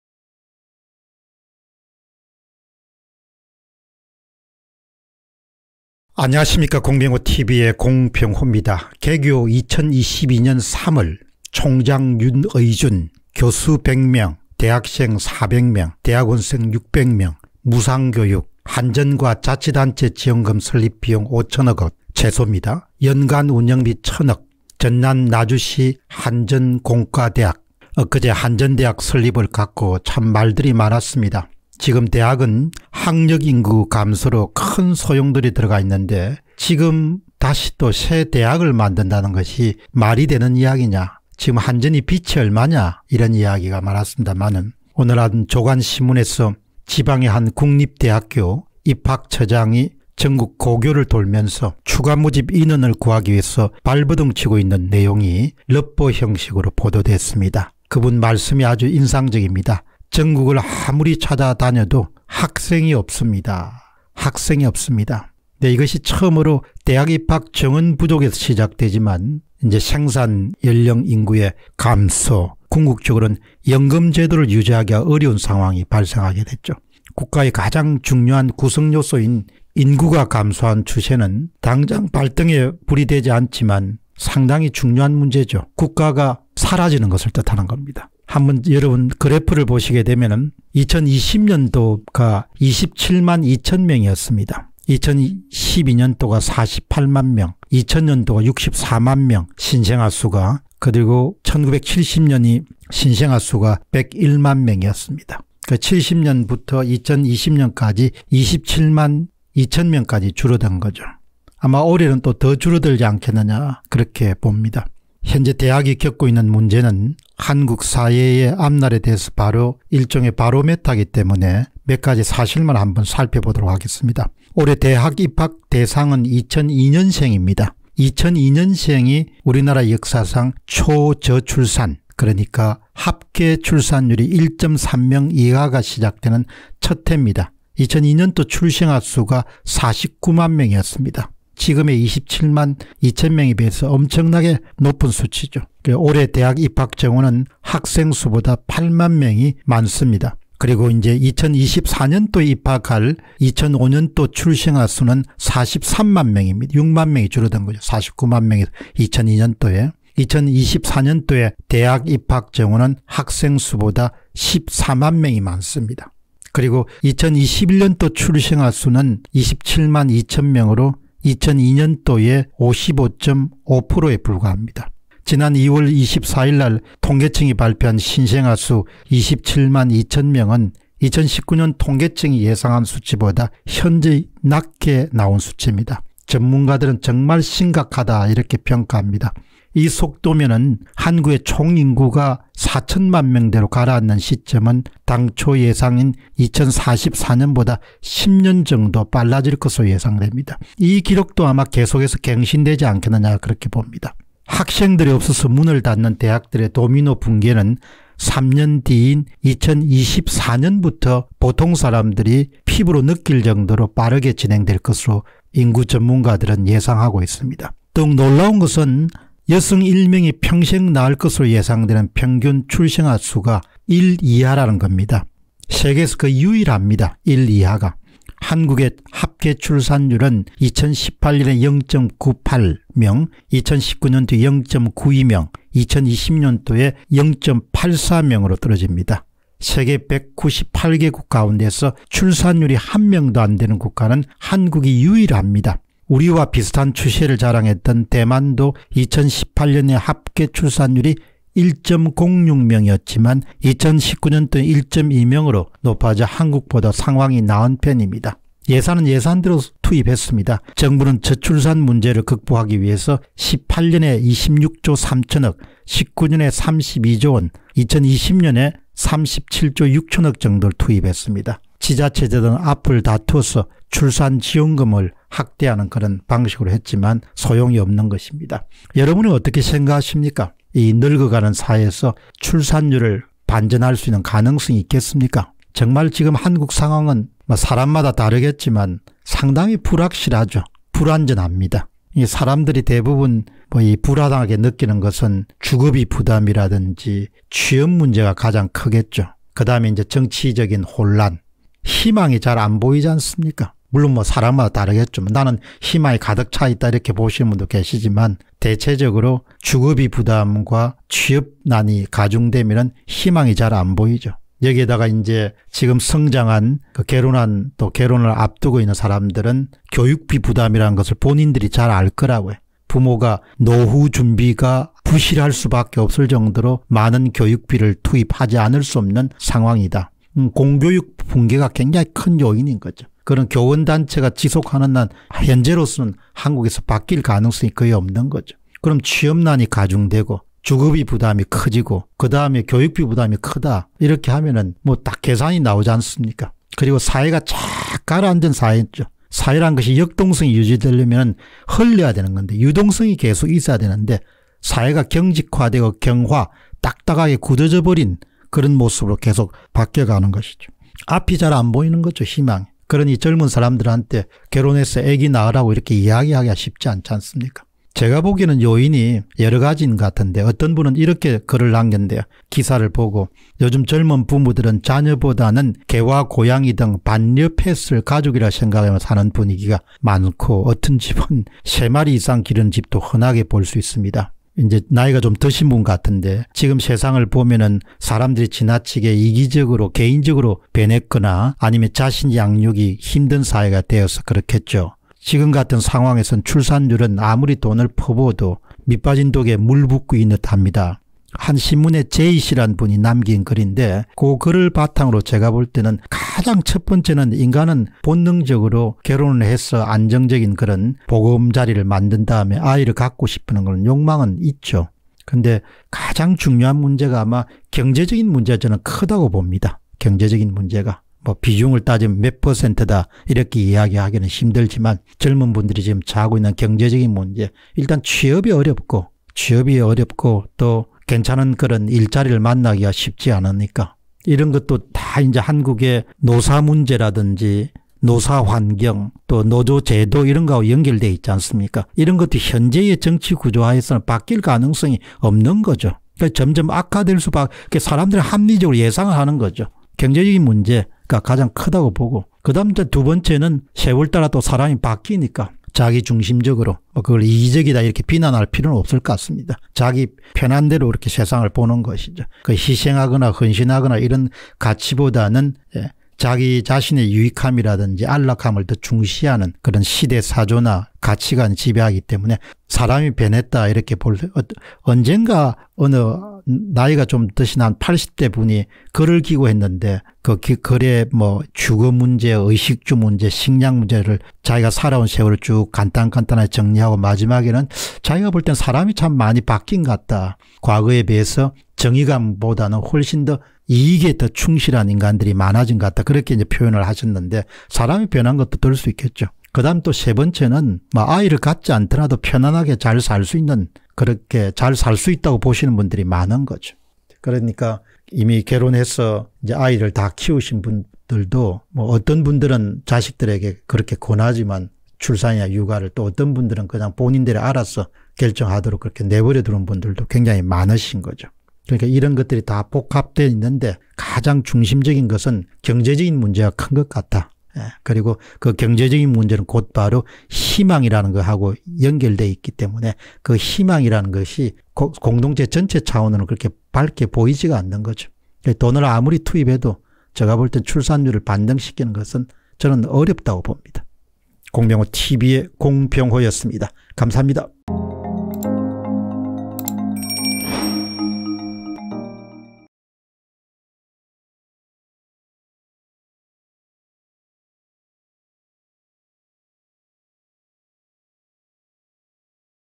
학생 찾아 삼만리 / 정말 큰 문제... / "대학정원의 문제가 아니라 연금제도 등이 유지될 수 ..." [공병호TV]
Bitcoin : 3MTW849AkP2ftLeSK2teZHMmzLu7Z83s9U